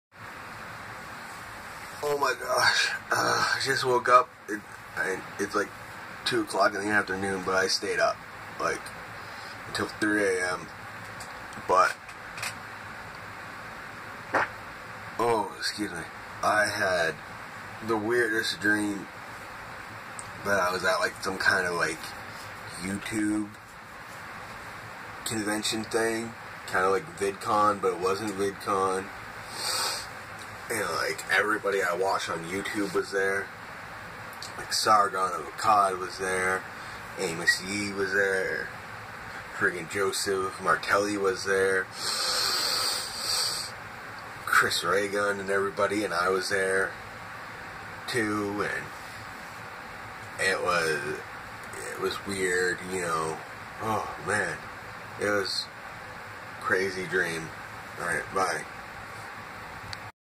Had a weird dream about a convention.
I had a weird dream that I was at a convention similar to VidCon where all my favorite YouTubers and some people who I don't like we're there everybody from Joseph Martelli to Amos Yee we're at the convention all getting along.  It was a good dream.